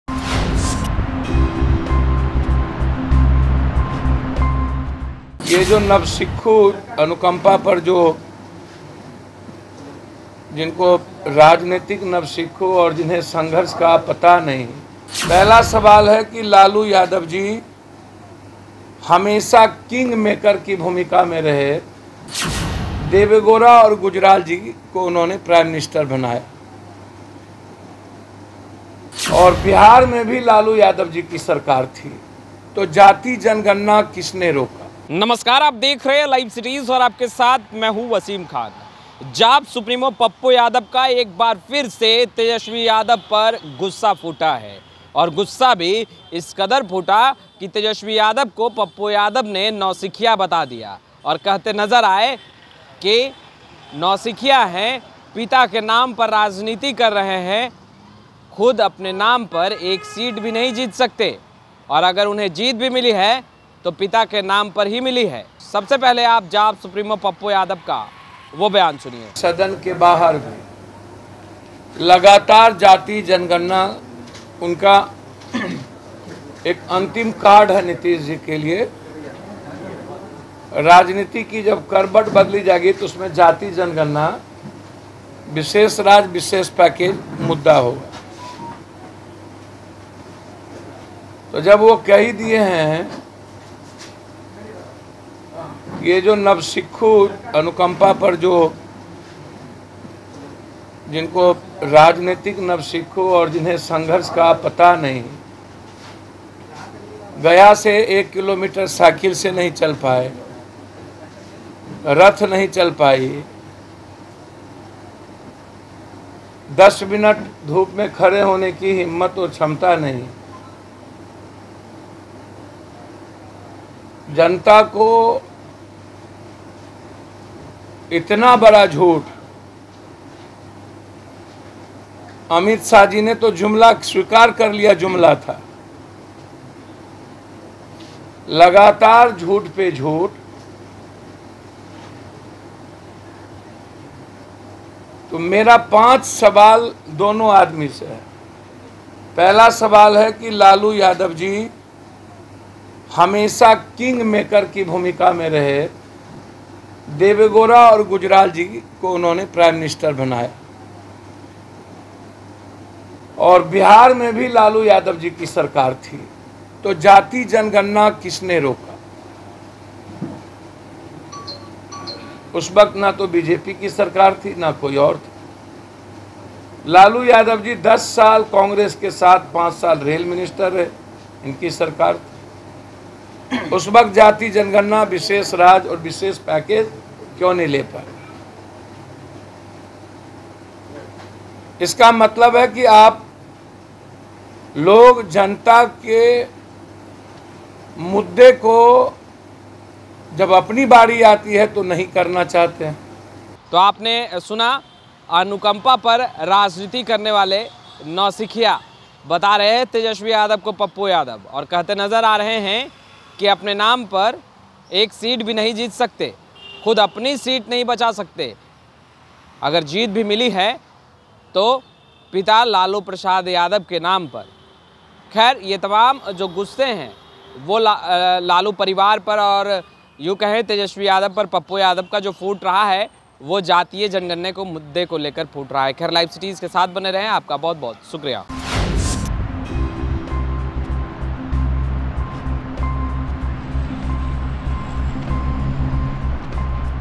Pappu Yadav ने Tejashwi Yadav को जमकर सुनाया, बोले- Lalu Yadav के नाम पर कर रहा राजनीति | Bihar News
Pappu Yadav ने Tejashwi Yadav को जमकर सुनाया, बोले- Lalu Yadav के नाम पर कर रहा राजनीति | Bihar News | LiveCities